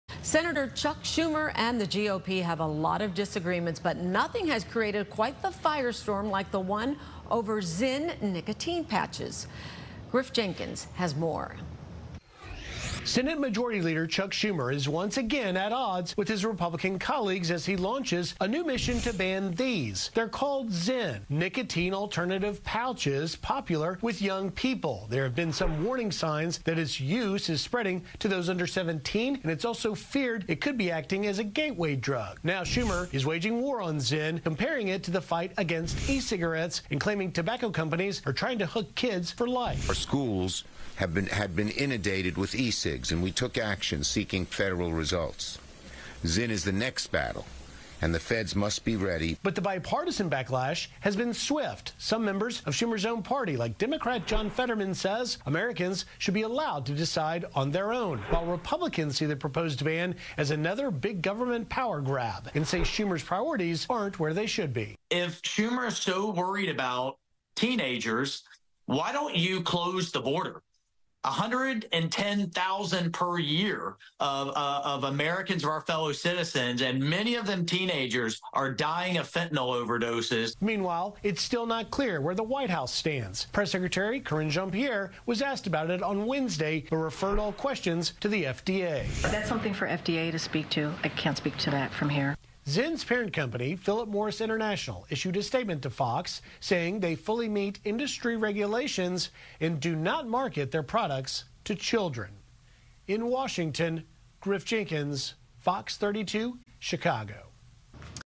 ZYN nicotine pouch ban being mulled over by Congress
Sen. Chuck Schumer (D-NY) and Republicans have a lot of disagreements but nothing has created a firestorm like the one over ZYN nicotine pouches.